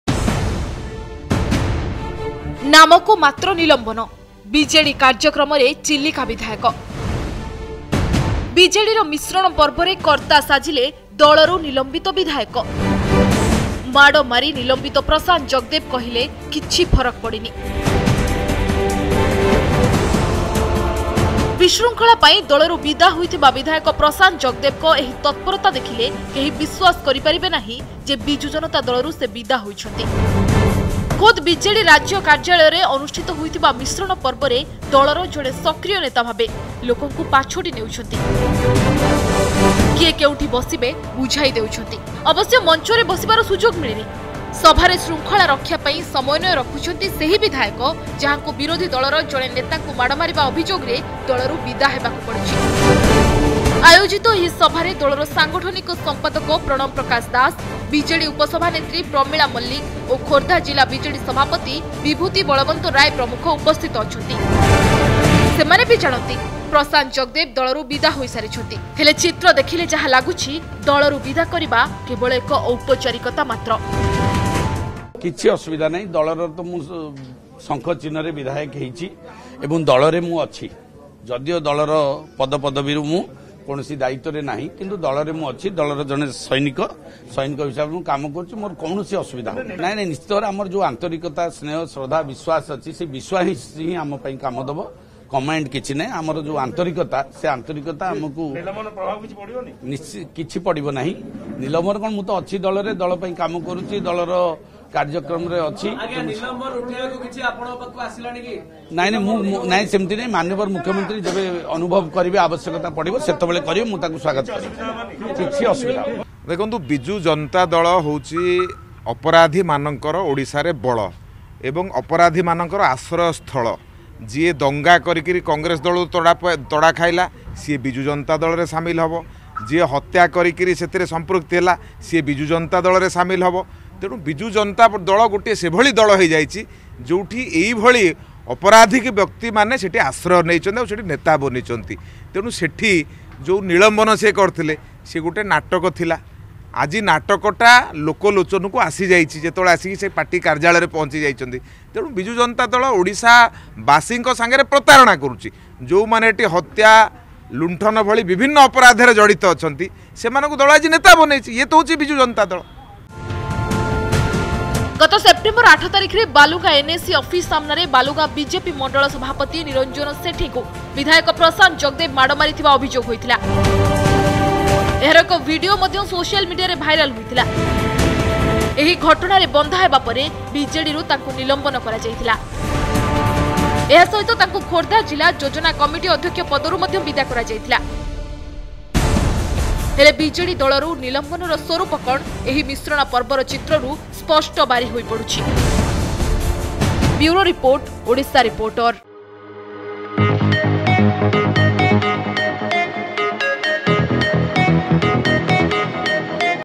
ନାଟକର ନାଁ ନିଲମ୍ବନ ଅଭିନେତା ପ୍ରଶାନ୍ତ ଜଗଦ୍ଦେବ | Odisha Reporter
ବିଶୃଙ୍ଖଳା ପାଇଁ ଦଳରୁ ବିଦା ହୋଇଥିବା ବିଧାୟକ ପ୍ରଶାନ୍ତ ଜଗଦ୍ଦେବଙ୍କ ଏହି ତତ୍ପରତା ଦେଖିଲେ କେହି ବିଶ୍ବାସ କରିପାରିବେ ନାହିଁ ଯେ, ବିଜୁ ଜନତା ଦଳରୁ ସେ ବିଦା ହୋଇଛନ୍ତି। ଖୋଦ୍, ବିଜେଡି ରାଜ୍ୟ କାର୍ଯ୍ୟାଳୟରେ ଅନୁଷ୍ଠିତ ହୋଇଥିବା ମିଶ୍ରଣ ପର୍ବରେ ଦଳର ଜଣେ ସକ୍ରିୟ ନେତା ଭାବେ, ଲୋକଙ୍କୁ ପାଛୋଟି ନେଉଛନ୍ତି, କିଏ କେଉଁଠି ବସିବେ ବୁଝାଇ ଦେଉଛନ୍ତି।